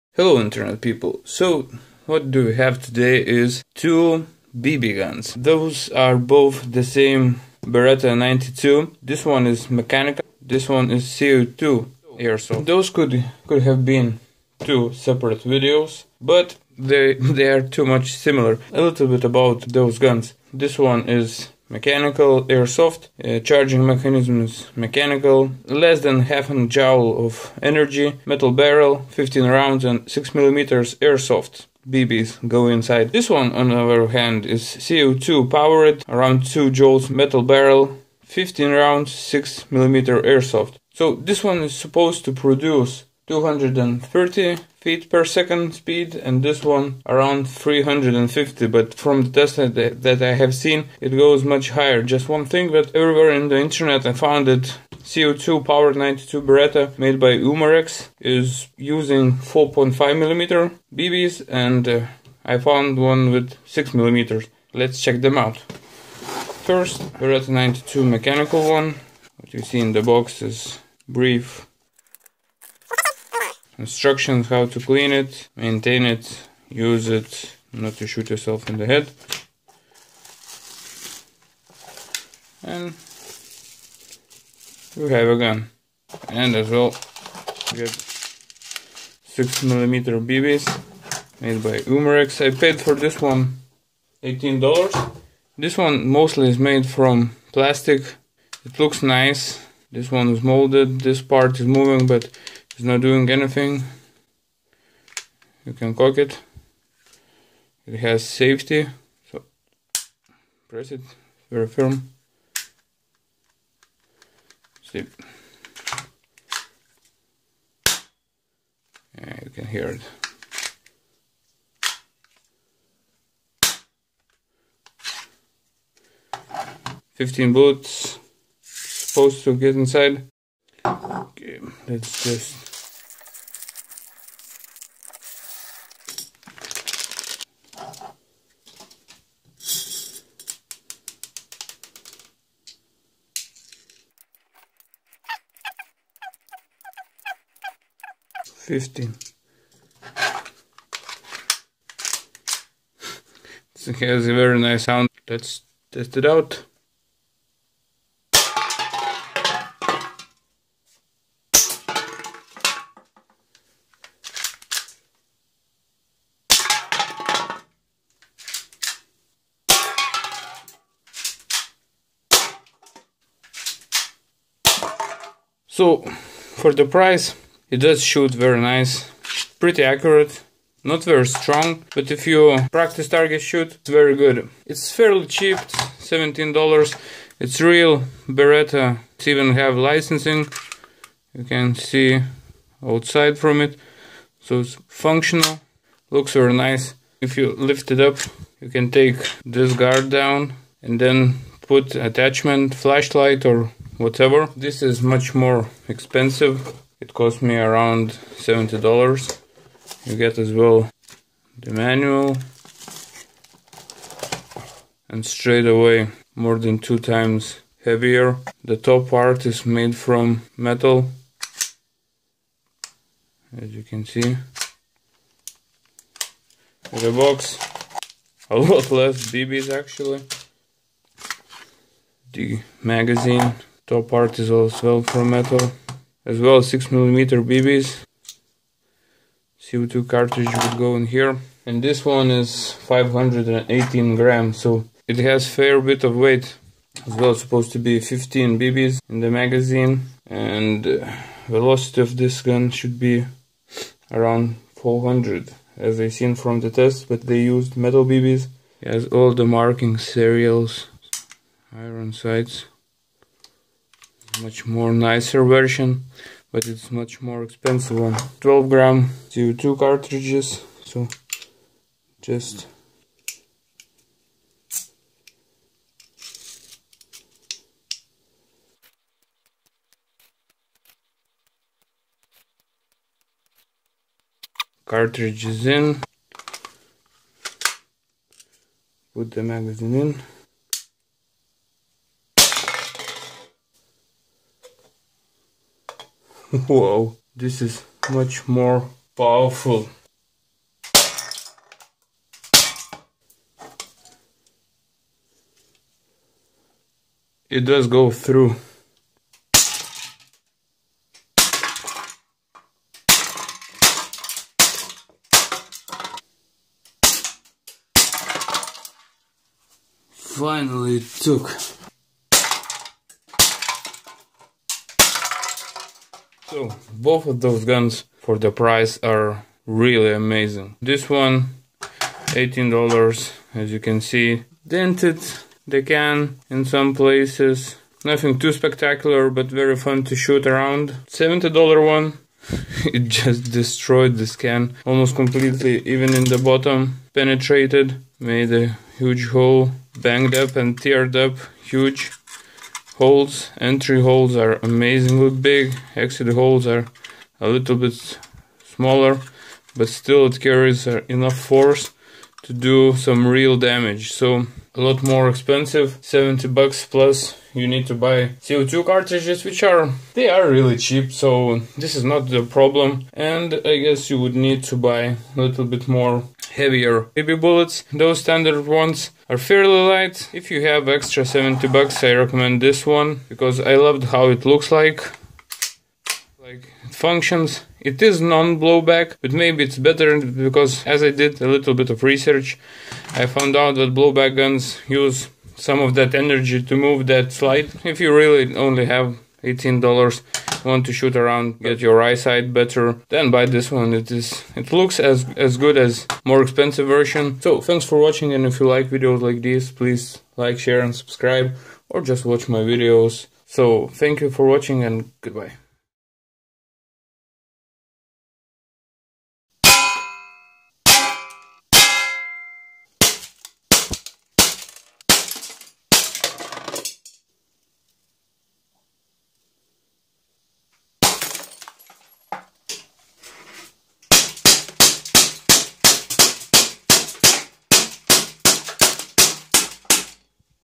umarex Beretta 90two airsoft mechnical and CO2 pistol unboxings
Nice airsoft guns. Cheap mechanical one, and bit more expensive CO2. Bought at gunfire.pl